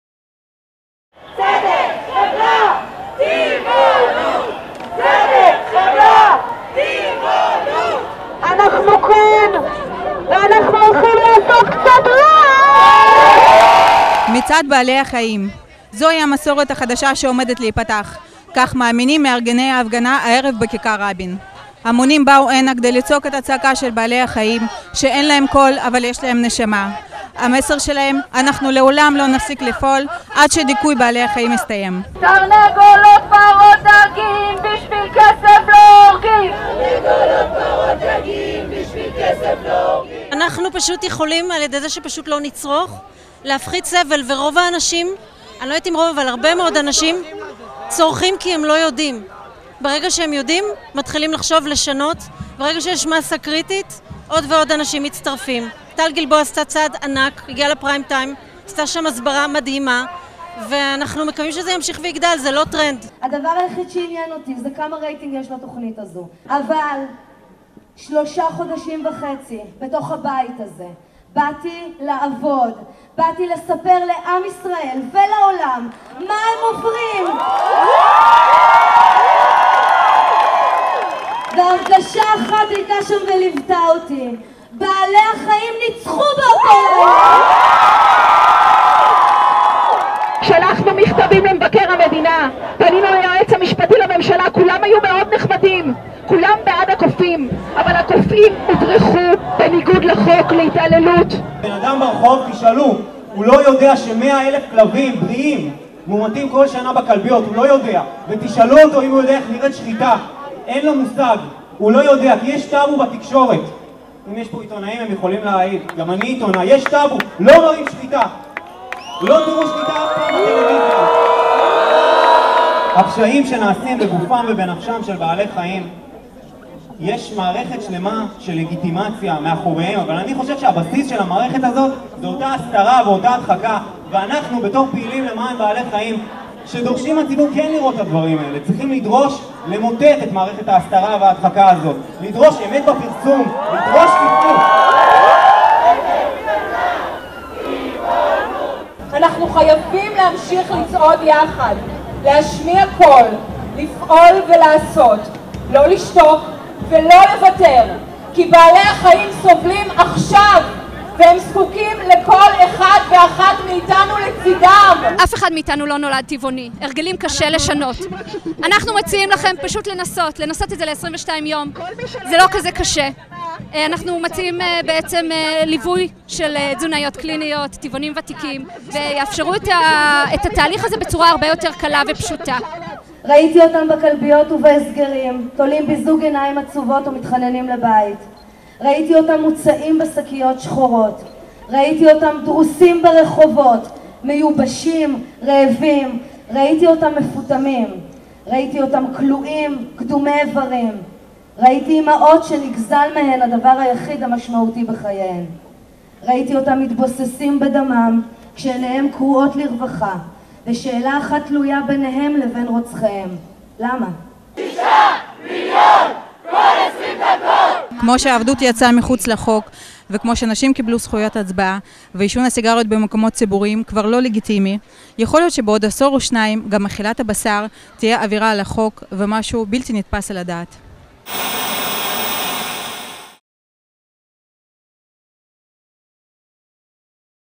Animal Rights Parade 2014
The second march for the rights of animals held on Saturday, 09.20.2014, against the continued oppression of animals on farms, experimental laboratories, sheds and streets.